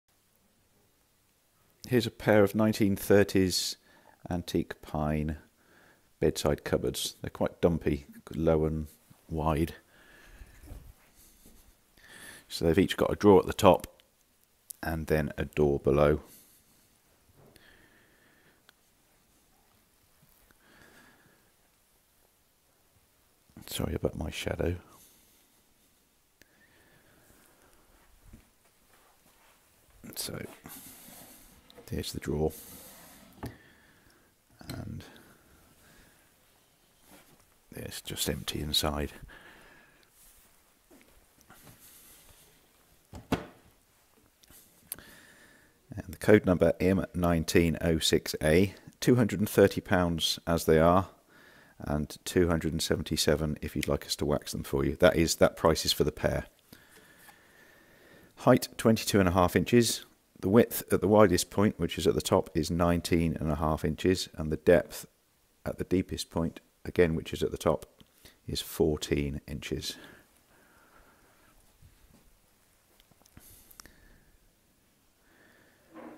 Pair of Small Antique Bedside Cupboards - Pinefinders Old Pine Furniture Warehouse
A pair of 1930's small antique pine bedside cupboards. They are quite low and wide cupboards, each with a small drawer at the top and cupboard below.
Stripped to the bare wood, they are in very nice condition, now ready for painting or waxing.

Code number M1906A

Price as they are, in the bare wood £230.

Height 22.5", width 19.5", depth 14"